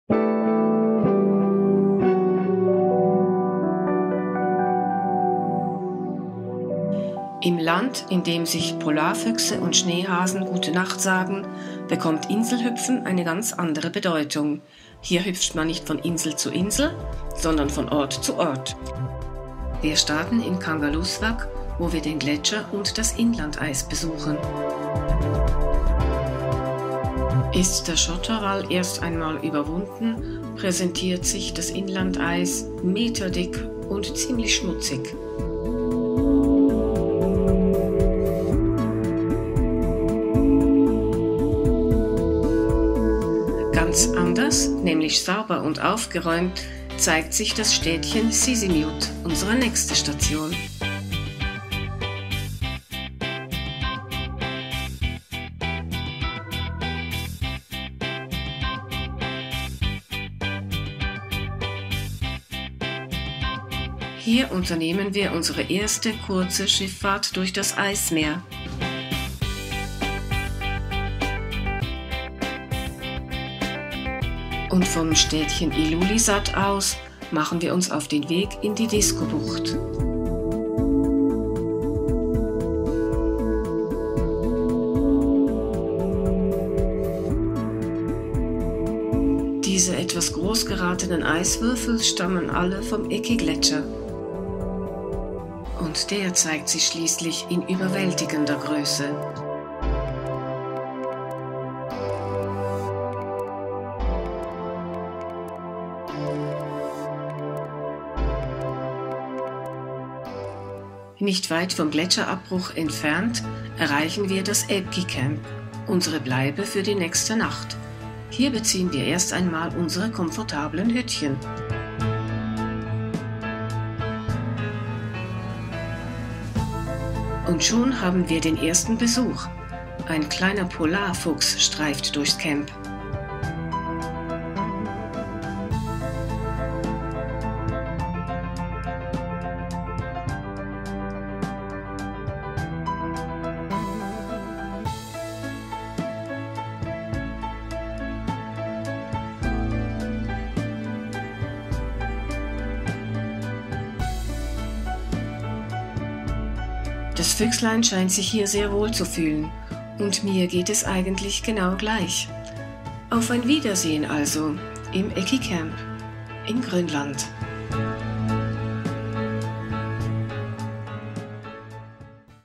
Grönland on the rocks
Die Impressionen einer kurzen Grönland-Reise.